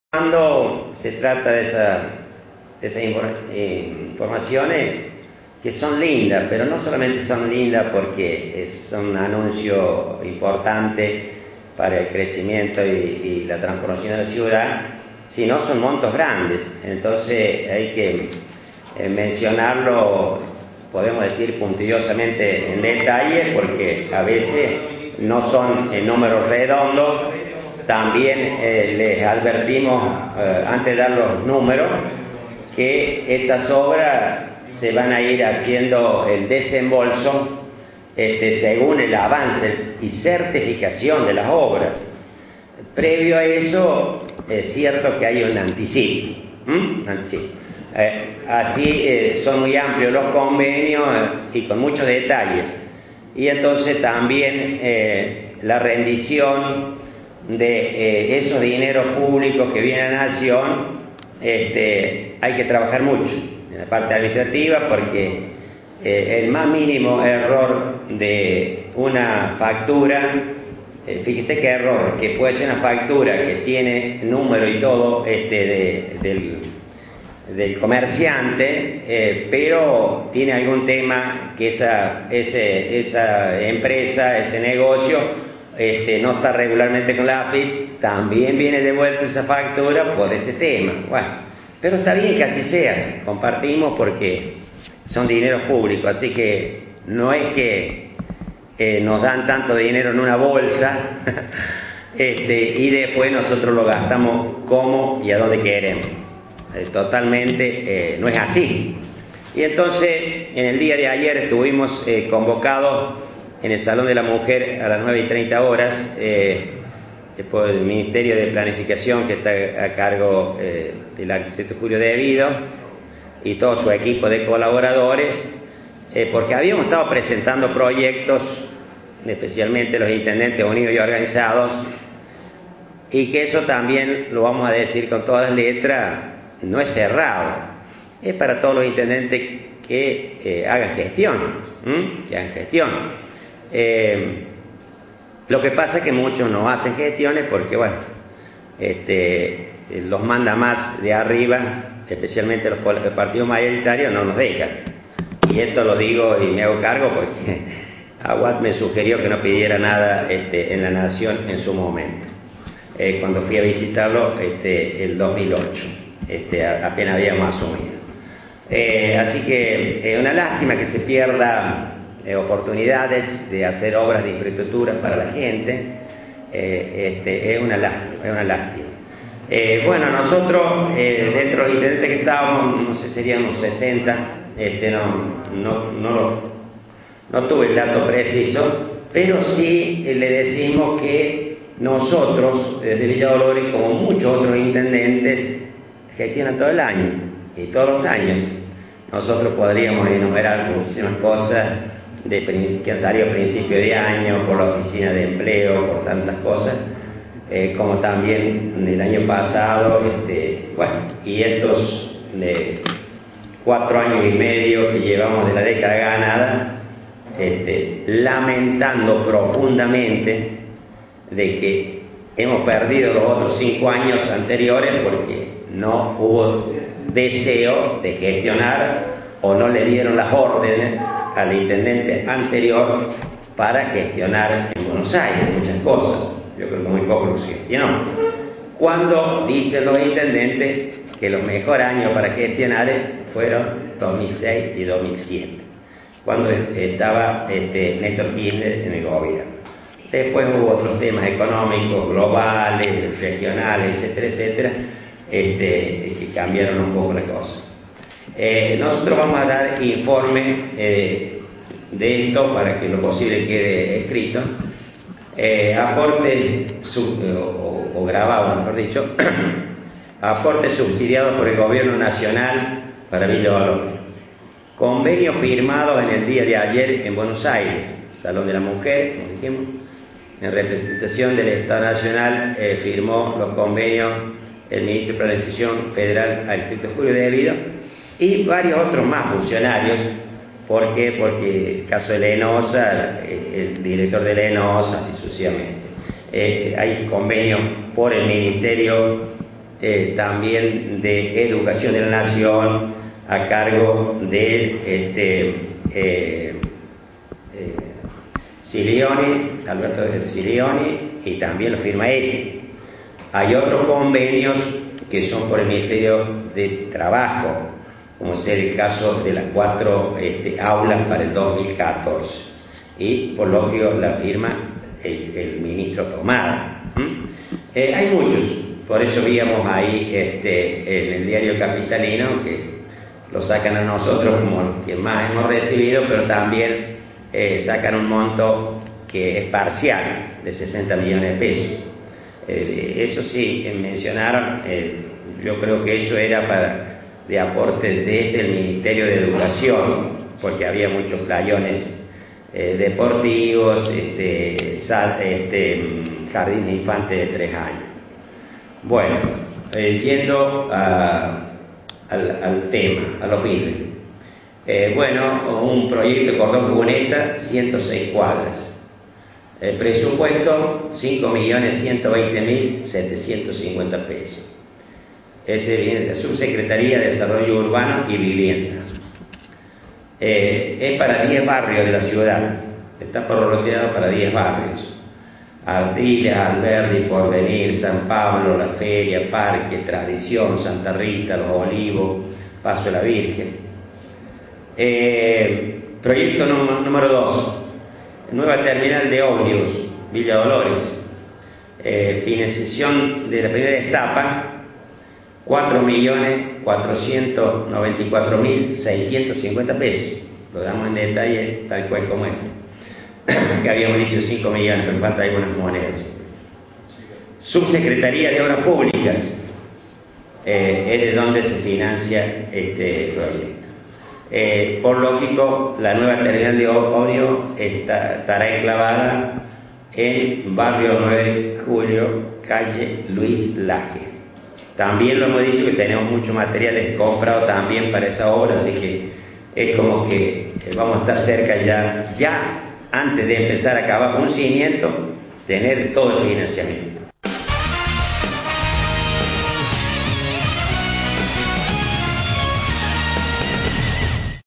IMPORTANTES APORTES ENTREGO LA NACION
El Intendente de Villa Dolores estuvo en Buenos Aires y recibió importantes aportes por parte del Gobierno Nación alrededor de 25 millones de pesos en Obras para nuestra ciudad.
En rueda de prensa informó sobre lo recibido.